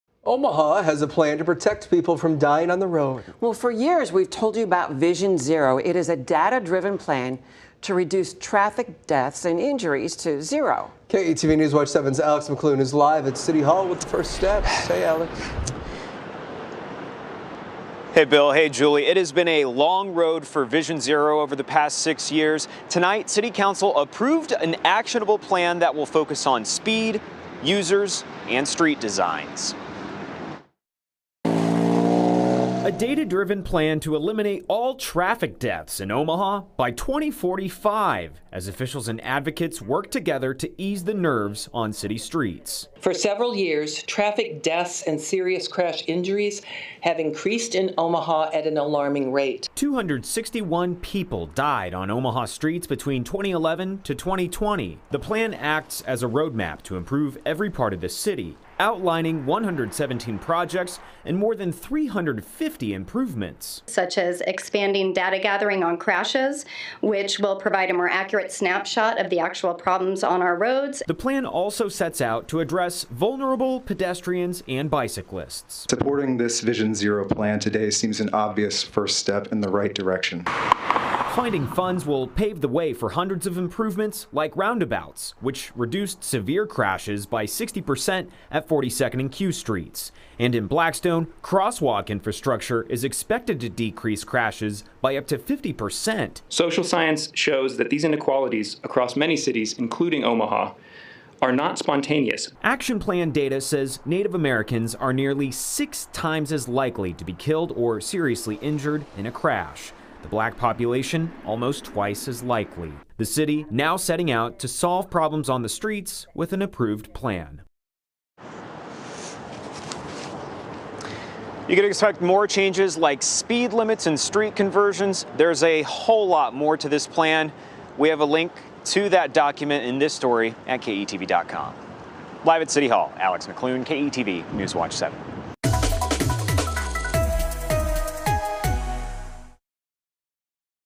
'First step in the right direction': Omaha adopts Vision Zero action plan to curb traffic deaths ...
Omaha adopts plan to reduce traffic deaths and serious injuries to zero by 2045.